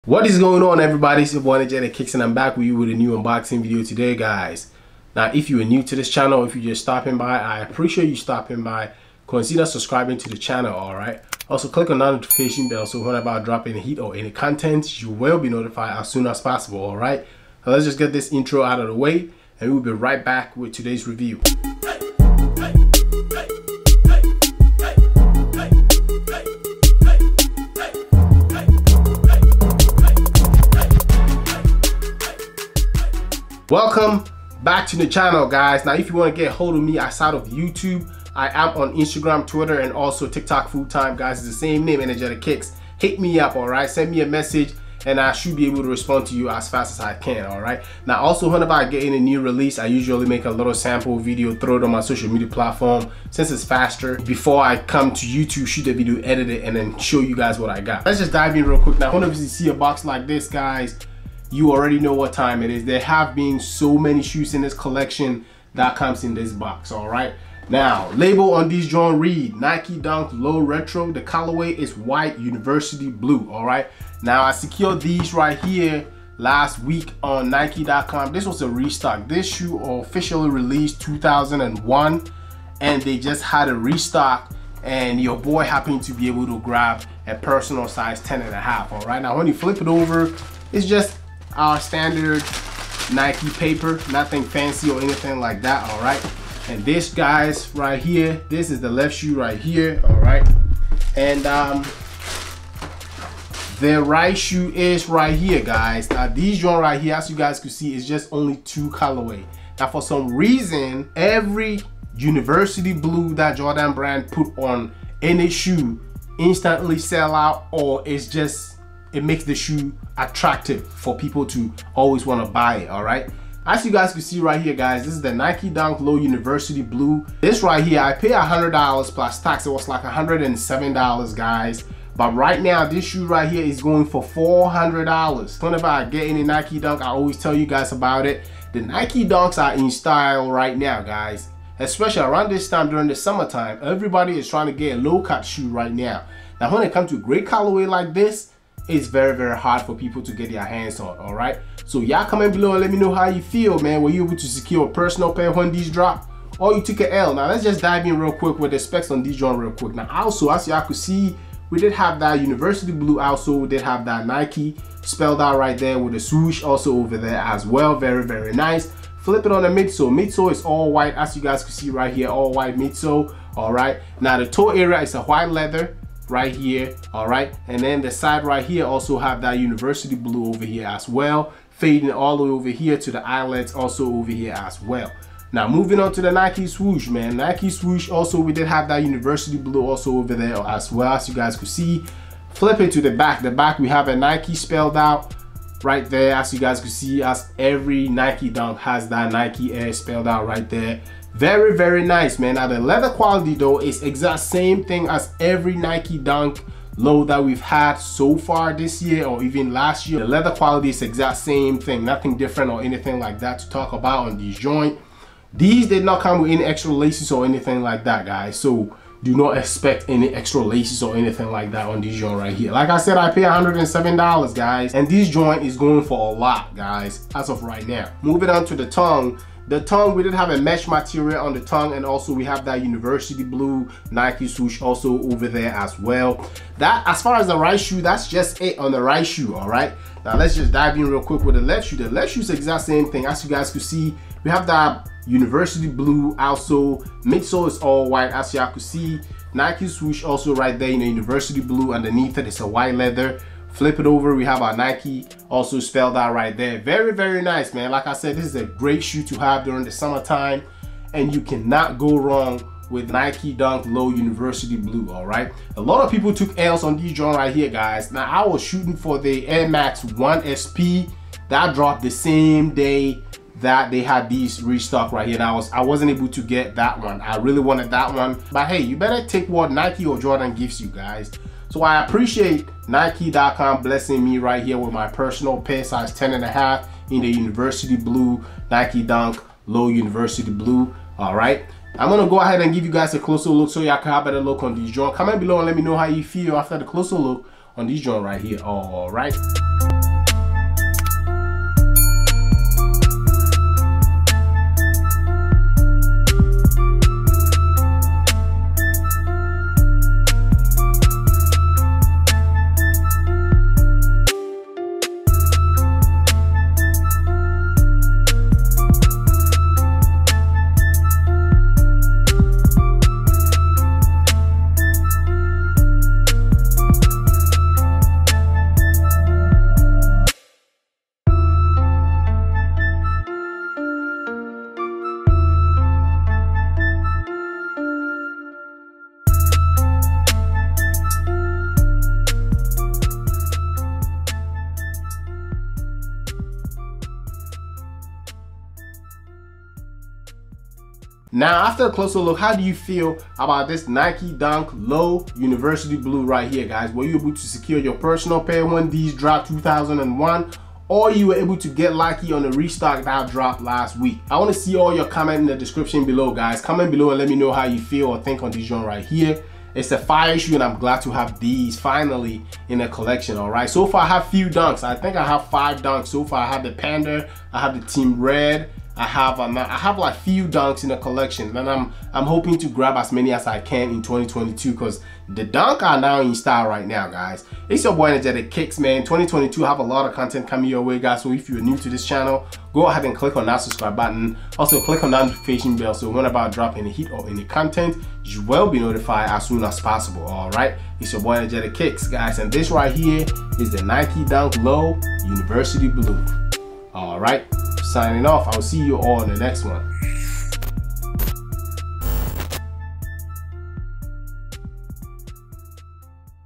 Nike Dunk Low Retro ( University Blue ) Review.
Nodding to the University of North Carolina Tarheels, the Nike Dunk Low UNC is constructed of a white leather upper with University Blue overlays and Swooshes. From there, a white and University woven Nike tongue label completes the design.

🙌🏿 Social Media 🙌🏿